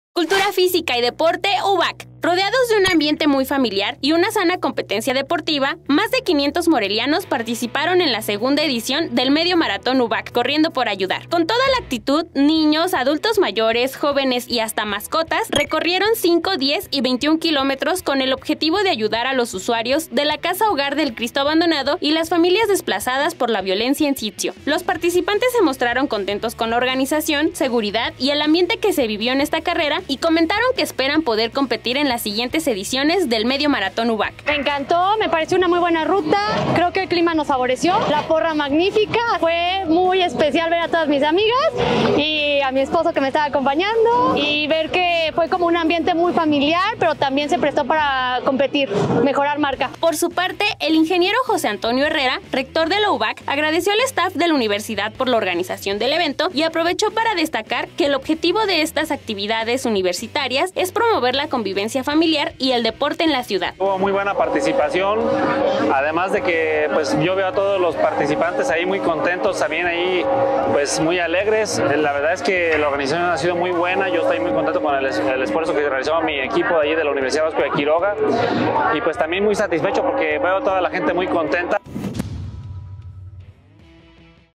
Familias morelianas corrieron por ayudar en el 2º Medio Maratón UVAQ
Con toda la actitud, niños, adultos mayores, jóvenes y hasta mascotas, recorrieron 5, 10 y 21 kilómetros en el 2o Medio Maratón UVAQ.